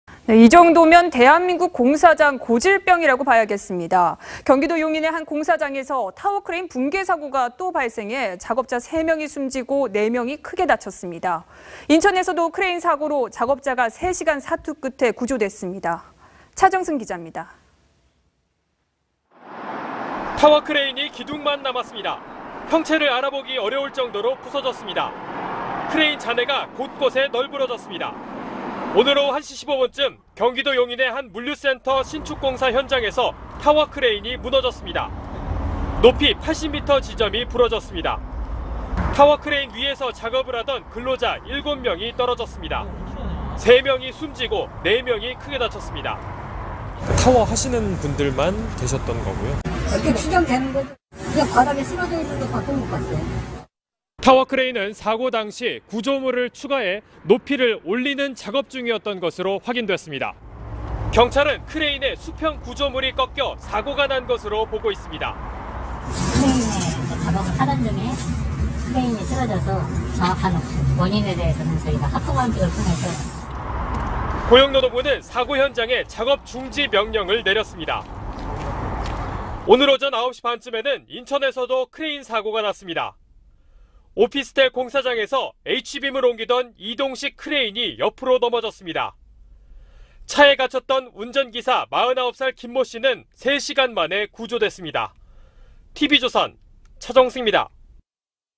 공사장 크레인 사고 잇따라…8명 사상
이 정도면 대한민국 공사장 고질병이라고 봐야겠습니다. 경기도 용인의 한 공사장에서 타워크레인 붕괴사고가 또 발생해 작업자 3명이 숨지고, 4명이 크게 다쳤습니다. 인천에서도 크레인 사고로 작업자가 3시간 사투 끝에 구조됐습니다.